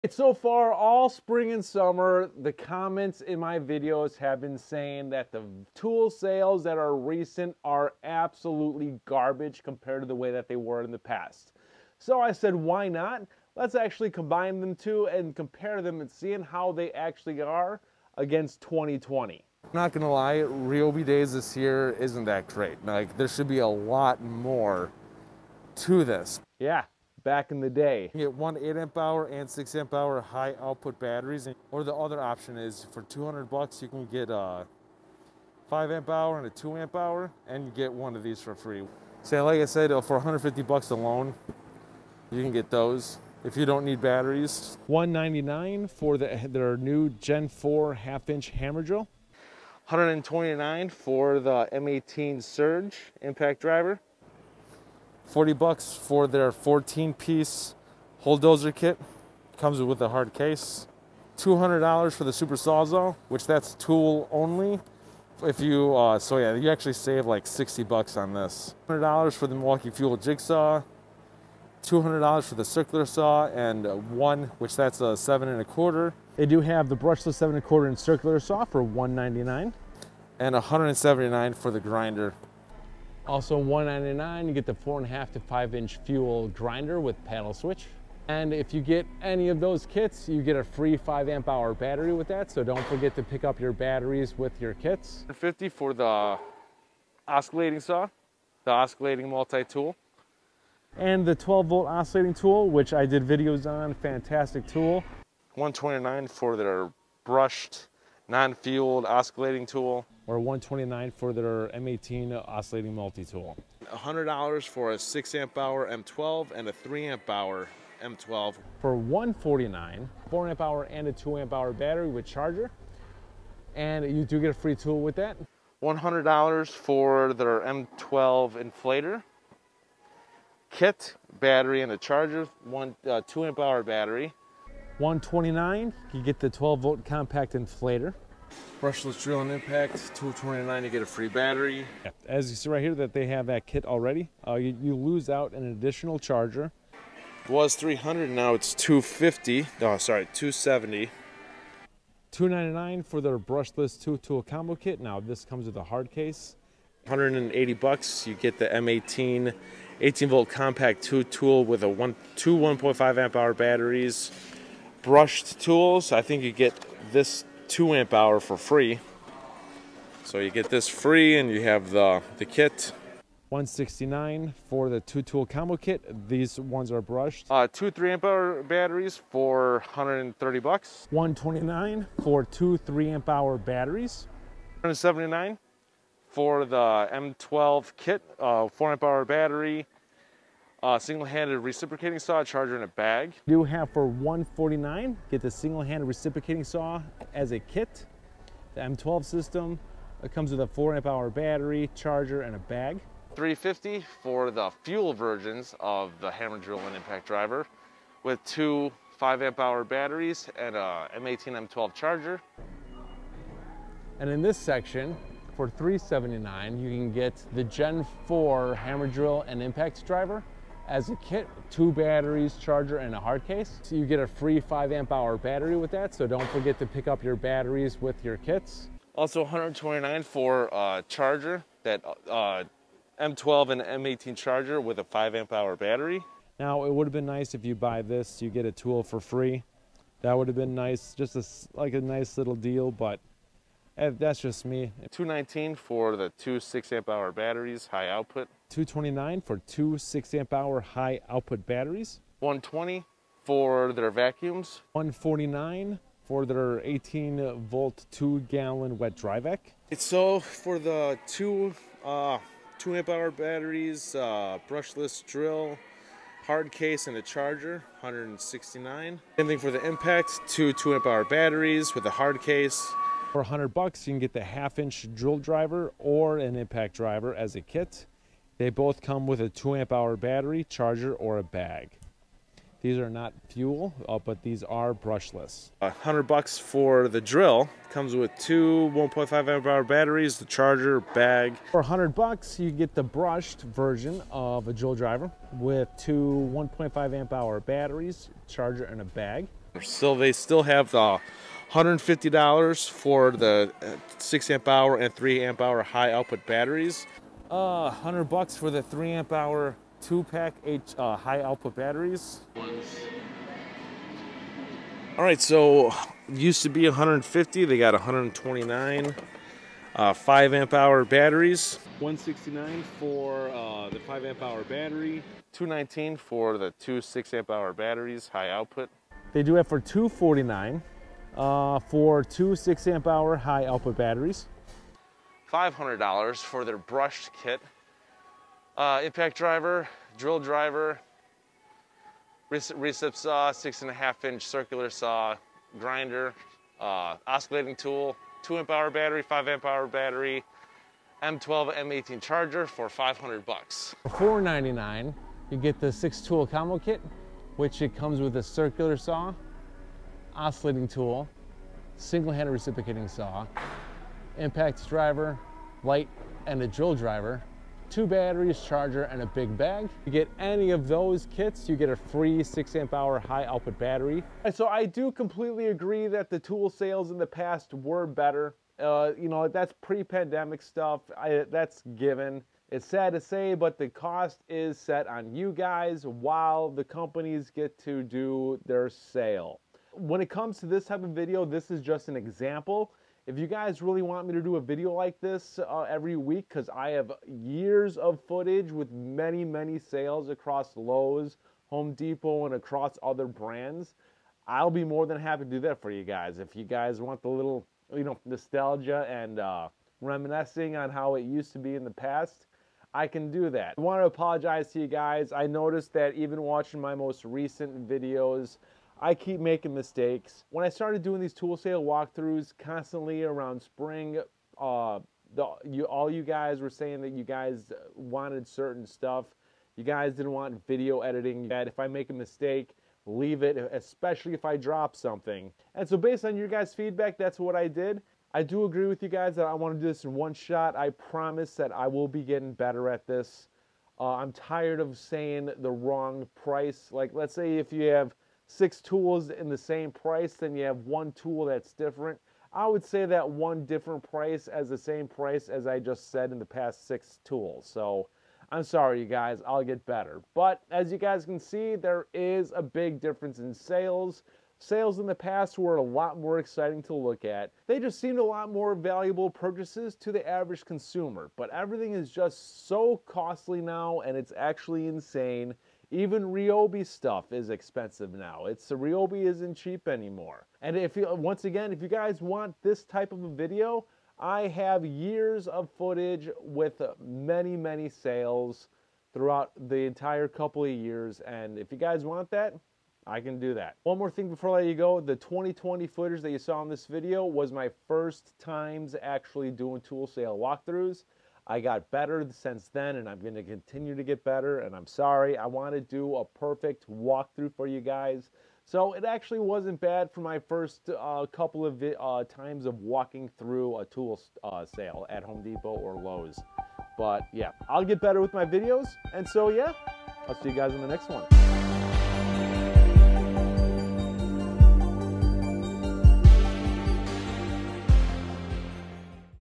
2020 vs 2023 Milwaukee Tool Sale Prices at HOME DEPOT!
This is an example of a video that I can make through out all brands. We can compare tool prices from the previous tool sales with Dewalt, Kobalt, Ryobi, Makita, RIDGID, Craftsman, FLEX and many others.

I thank you for checking out this video, lets find out how well these tools sales are and see what tool sales is best for you.  I am not apart of any other social media. Youtube is the onyl plateform this channel supports. Have a great day!